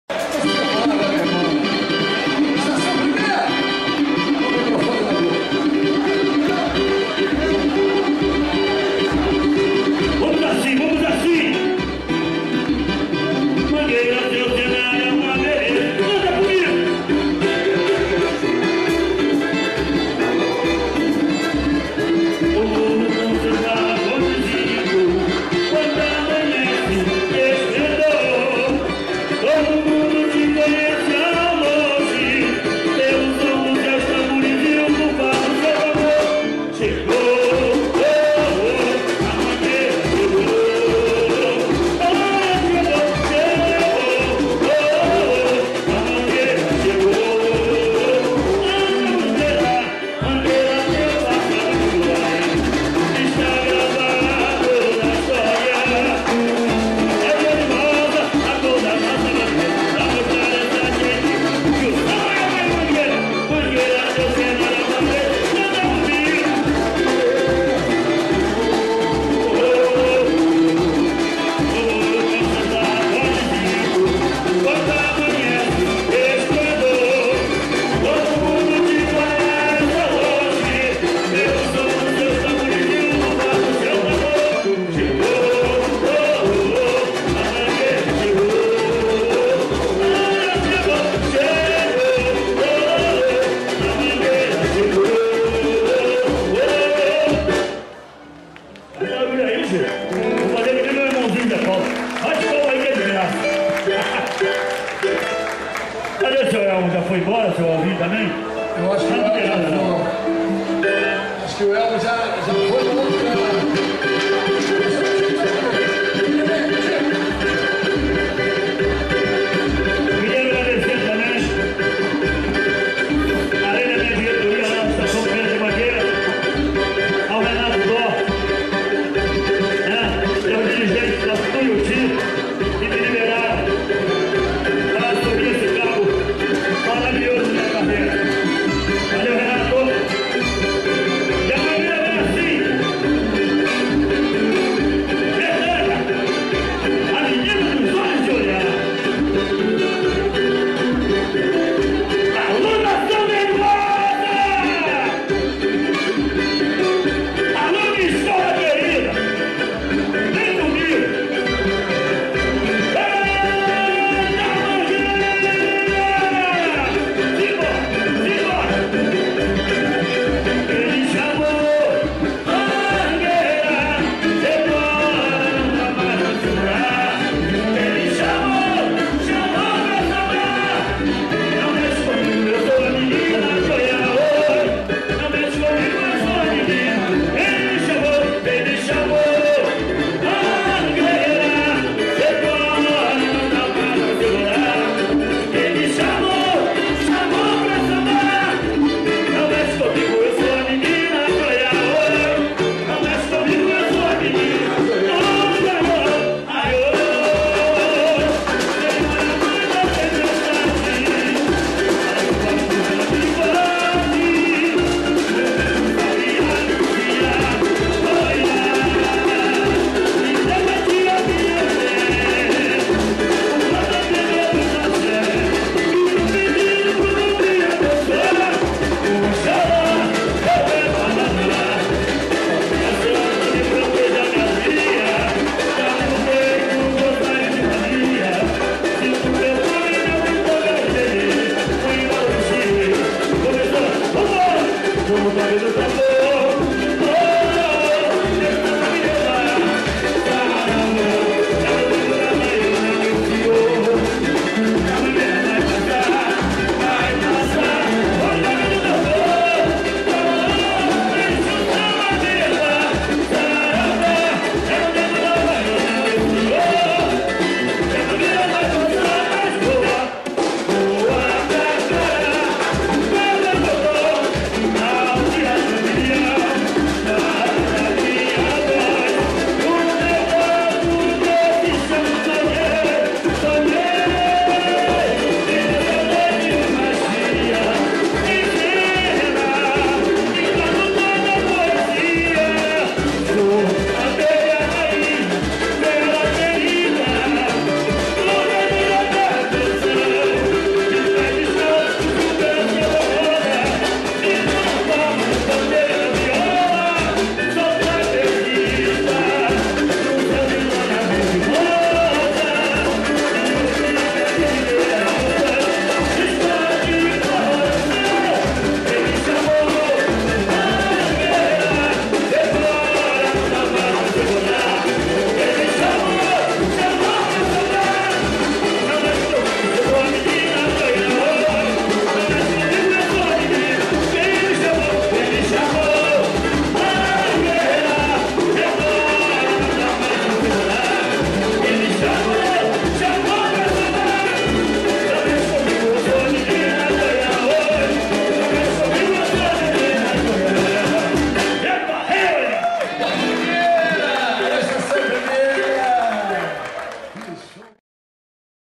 Estação Primeira de Mangueira - Botequim do Carnavalesco (06/10/2015)
Botequim do Carnavalesco no Cordão da Bola Preta (06/10/2015)
Apresentação do G.R.E.S. Estação Primeira de Mangueira
Produzido por: Rodney de Figueiredo